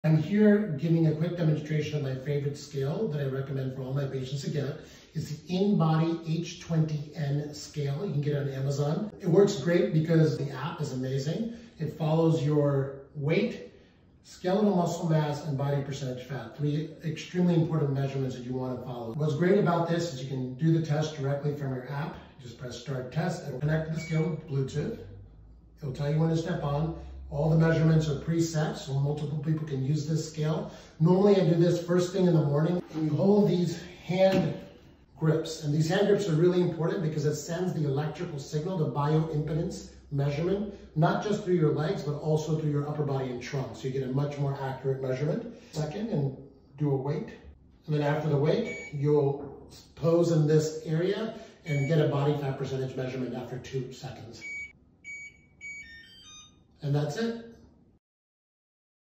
InBody H20N Scale
Let me tell you about this amazing health gadget that has been a game-changer for me  - InBody H20N Smart Full Body Composition Analyzer Scale.

It's not your ordinary bathroom scale, trust me! This smart device goes way beyond just measuring your weight. It's like having a personal health coach right at your fingertips! When I step on it, not only does it give me my weight, but it also provides detailed insights into my skeletal muscle mass and body fat percentage, which has been incredibly helpful for understanding my body composition.

One of the things I love the most is how easy it is to use. It connects effortlessly with my phone via Bluetooth, so I can keep track of my progress right on the app. It's so convenient to see how my workouts and healthy choices are paying off over time. I highly recommend giving the InBody H20N  Scale a try!